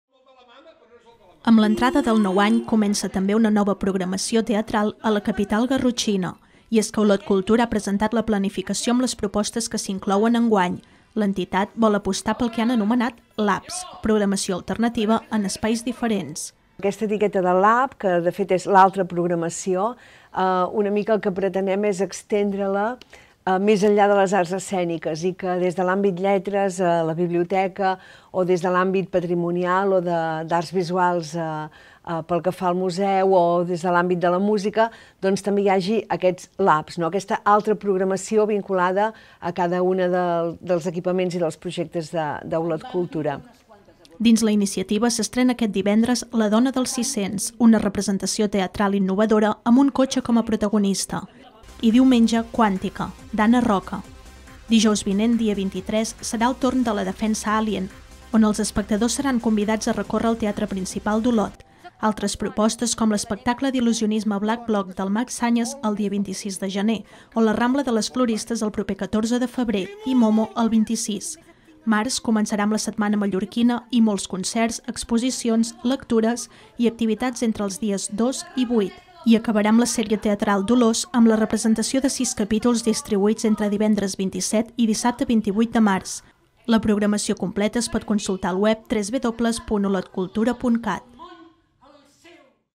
Olot Cultura aposta per una programació teatral alternativa i en espais diferents
Amb l’entrada del nou any comença també una nova programació teatral a la capital garrotxina, i és que Olot Cultura ha presentat la planificació amb les propostes que s’inclouen enguany. L’entitat vol apostar pel que han anomenat ‘LAPS’, programació alternativa en espais diferents.